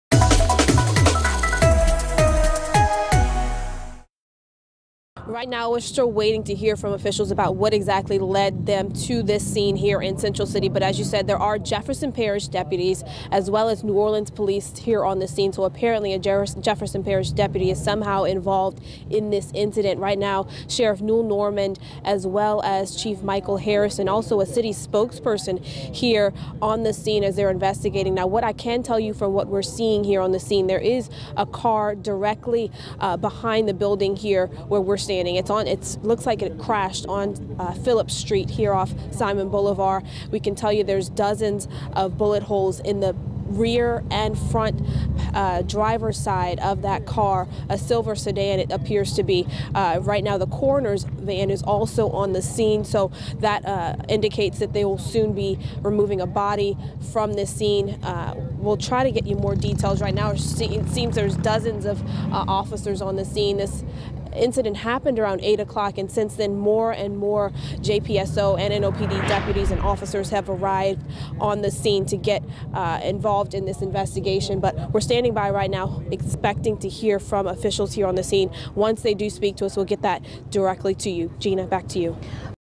NOPD, JPSO investigate scene in Central City
Several New Orleans police officers and Jefferson Parish deputies were sent to Central City to investigate an incident Monday night.